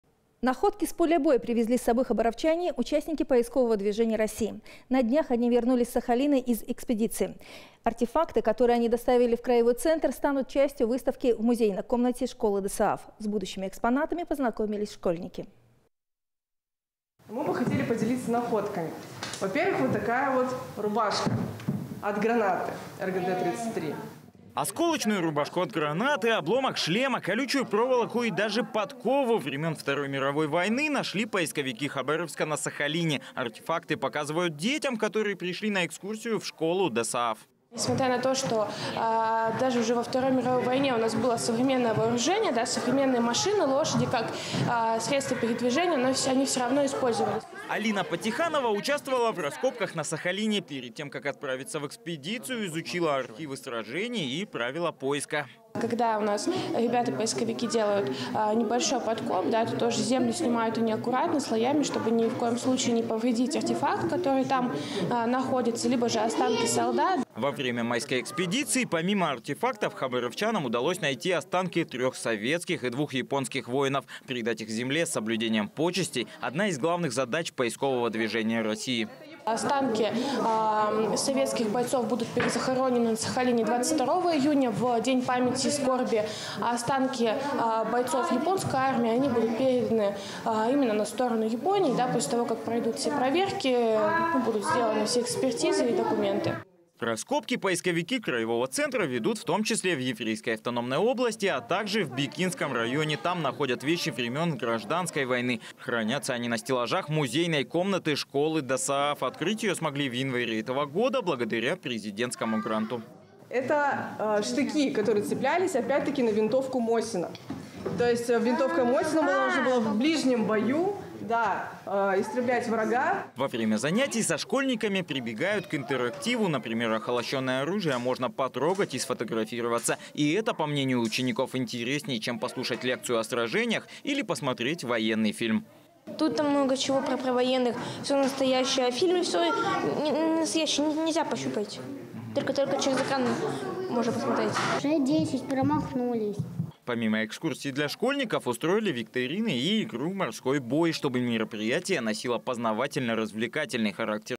Находки времён Второй мировой войны
Находки с поля боя привезли с собой хабаровчане — участники поискового движения России. На днях они вернулись с Сахалина из экспедиции. Артефакты, которые они доставили в краевой центр станут частью выставки в музейной комнате школы ДОСААФ. С будущими экспонатами познакомились школьники.